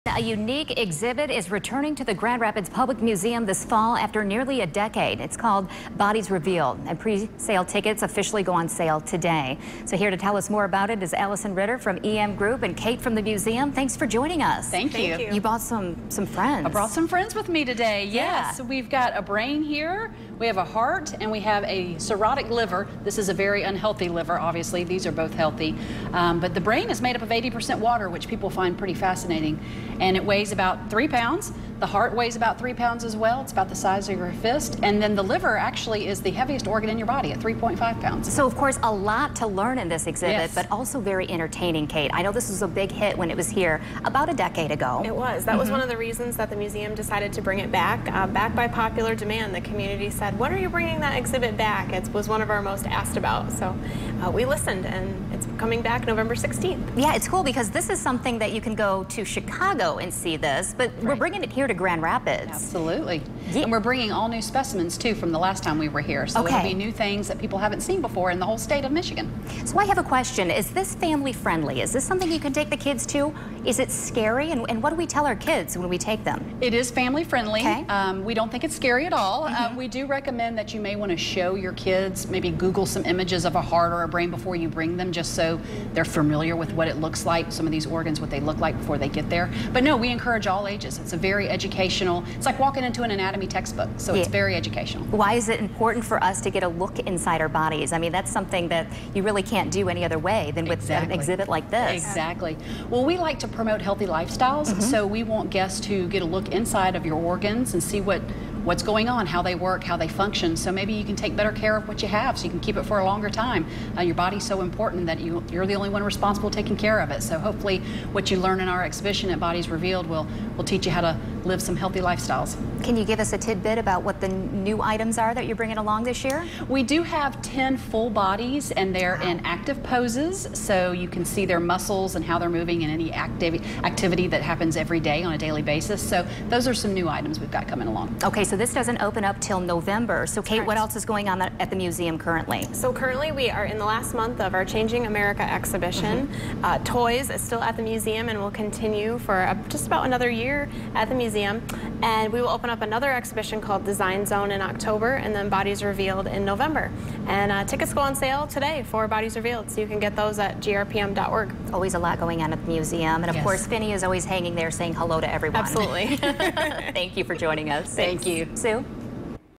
'Bodies Revealed' returns to Grand Rapids Public Museum
Tickets are now on sale for an exhibition showcasing actual bodies in active poses that will return to Grand Rapids this fall. (Sept. 18, 2019)